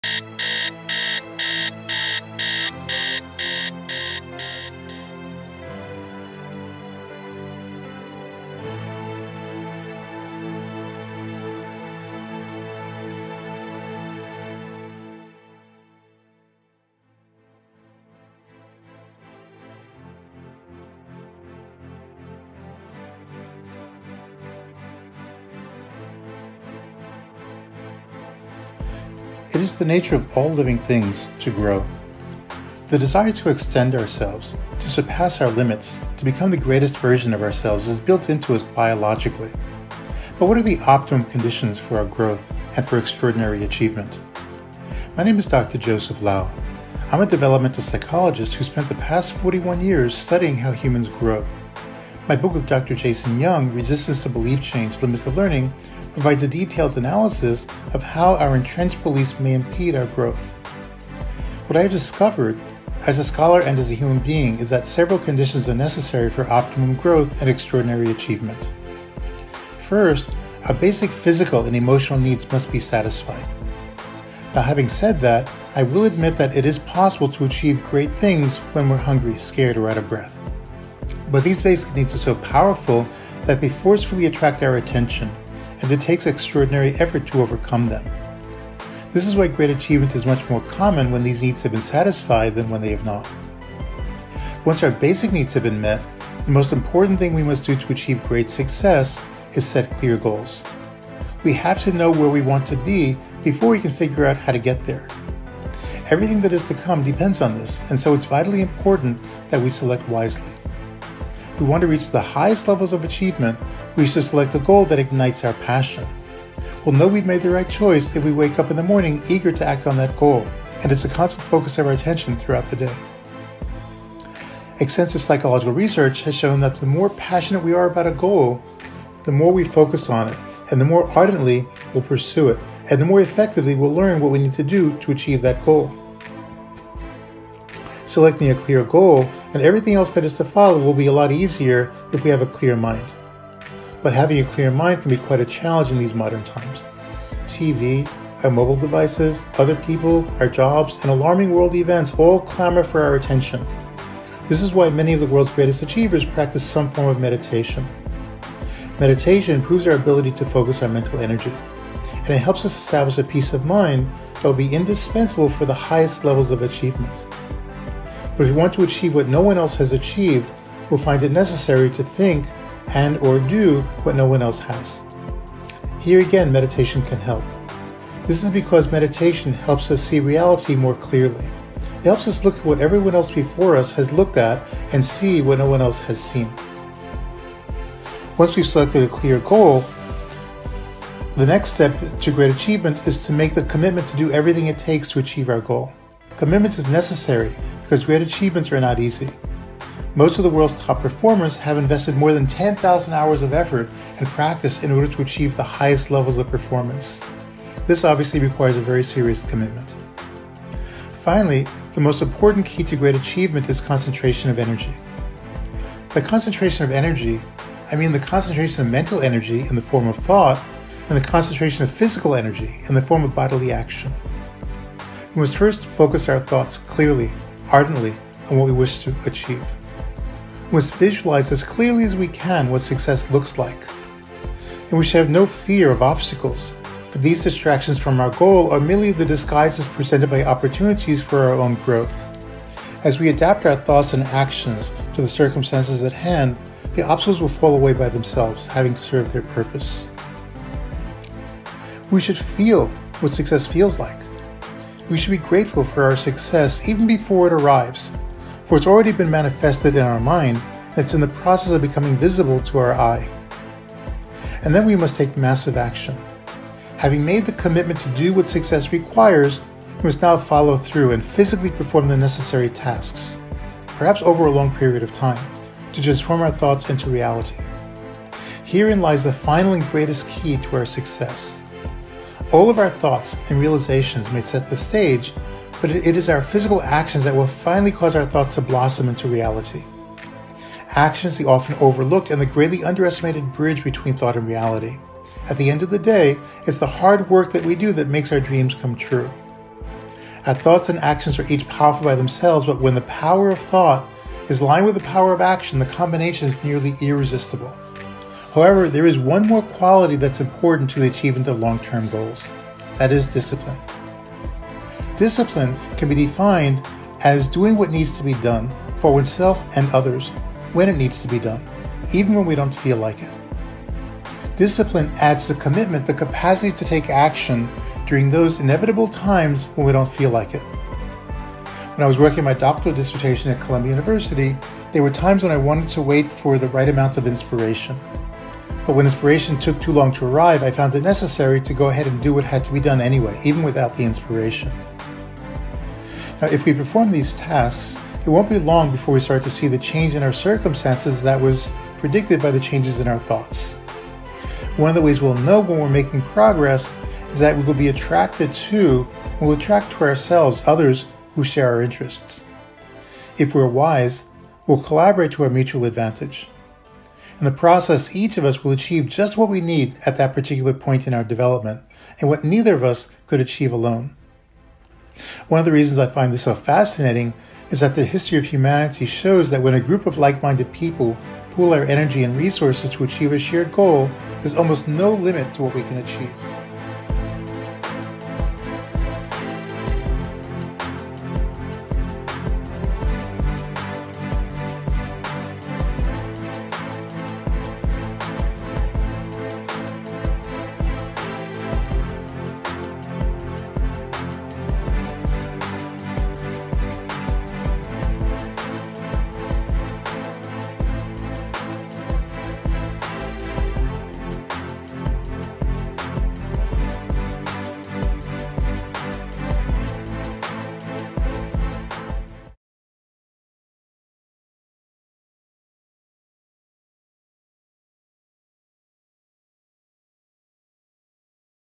Teaser
This video is a teaser, created by Jermaine Brantley, for a larger movie about the life of Dr. Joseph Lao.